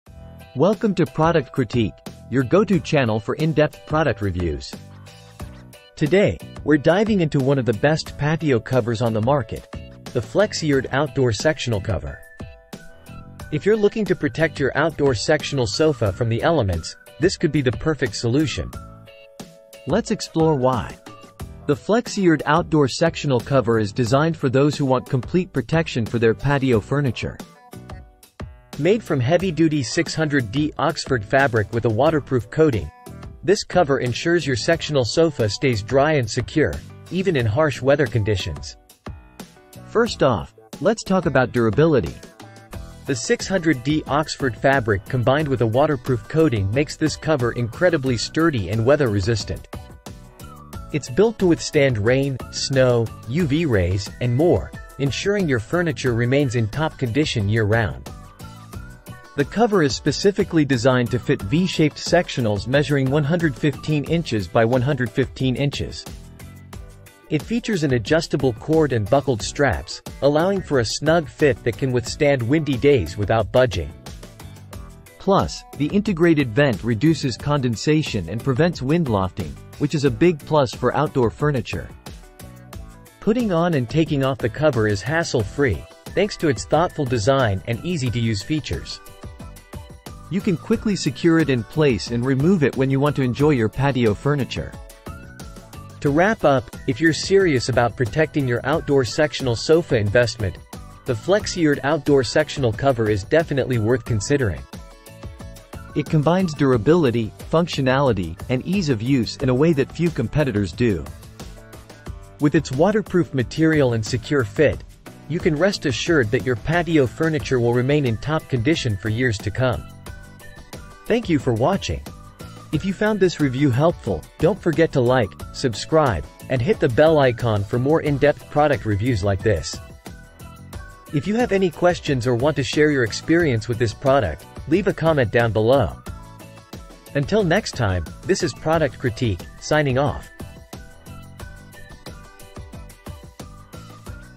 🌿 Flexiyard Outdoor Sectional Cover | Best Patio Covering 🏡
Amazon Product Name : Flexiyard Outdoor Sectional Cover

Explore the ultimate guide to finding the Best Patio Covering for your outdoor space. Whether you're looking to shield your patio from the sun's harsh rays or create a cozy outdoor retreat, choosing the right covering is essential. From stylish pergolas to versatile umbrellas and durable awnings, discover various options that enhance both functionality and aesthetics. Watch as we delve into the features, benefits, and installation tips of the Best Patio Covering options available today. Transform your patio into a comfortable oasis with our expert recommendations.